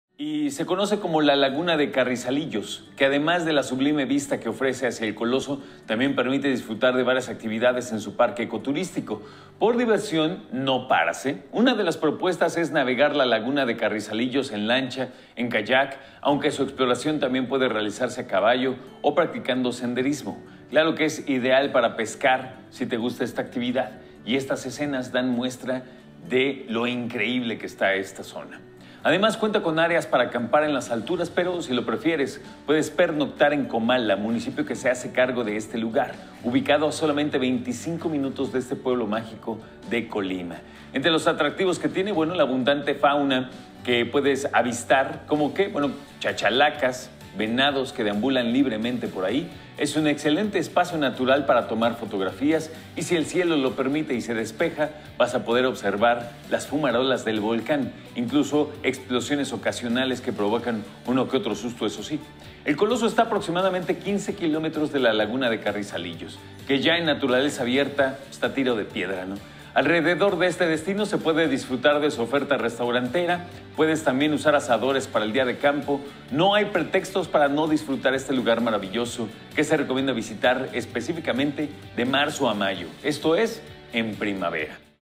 Laguna Carrizalillo, sublime vista a las faldas del volcán de Colima
Además de la sublime vista que ofrece hacia el coloso, también permite disfrutar de varias actividades en su parque Ecoturístico.